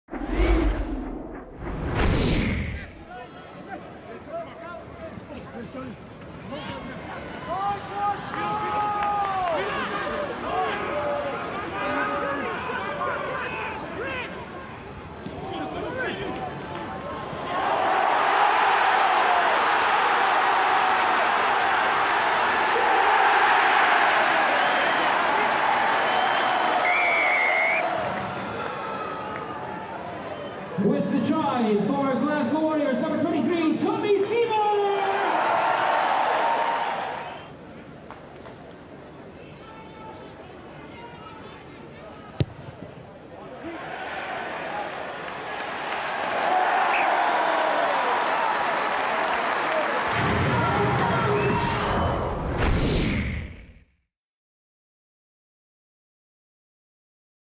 Great Tommy Seymour Break and Key Try for Glasgow Warriors v Cardiff Blues 6th Sept 2013
Great Tommy Seymour Break and Key Try for Glasgow Warriors v Cardiff Blues in their game in the RaboDirect PRO12 on Friday 6th September 2013 in Scotstoun Stadium, Glasgow .